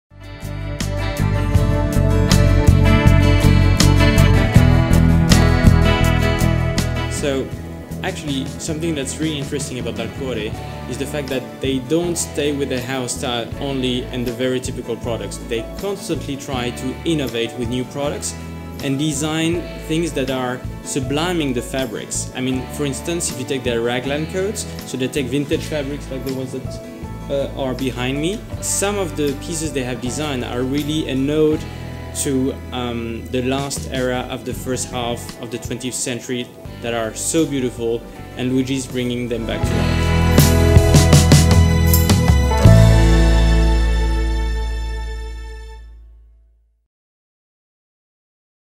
Paul Fournier to Naples for WhychooseDalcuore? - Part. 2
Our dearest friend Paul Fournier explains which are the main aspects for choosing the Sartoria Dalcuore.
We would like to thank him for the thoughts expressed on our team in whychooseDalcuore
We asked all our partners and all the characters who have a certain expertise in the Sartorial field to express their thoughts on the values, and on the main peculiarities of the Sartoria Dalcuore.
•Thanks Paul Fournier
🎥The new series
Video: Roby Baccante.